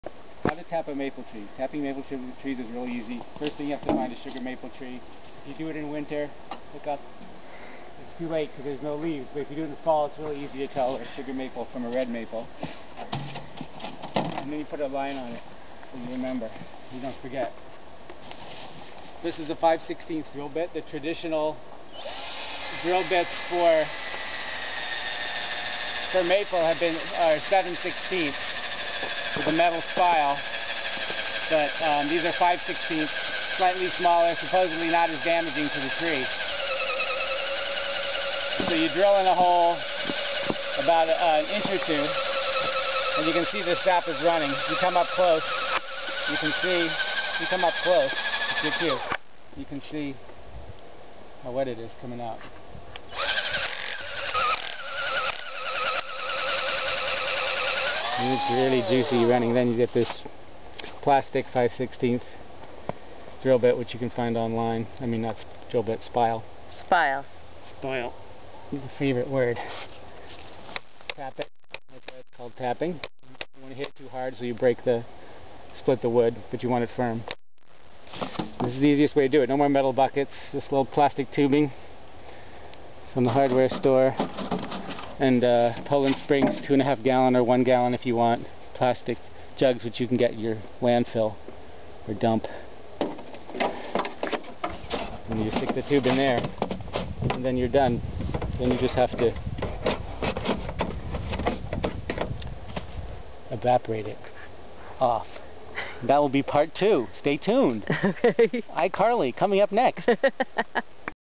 How to Make Maple Syrup - part 1
How to tap a sugar maple tree for maple syrup.  it's easy to do small scale on your own land, and get enough syrup for the year.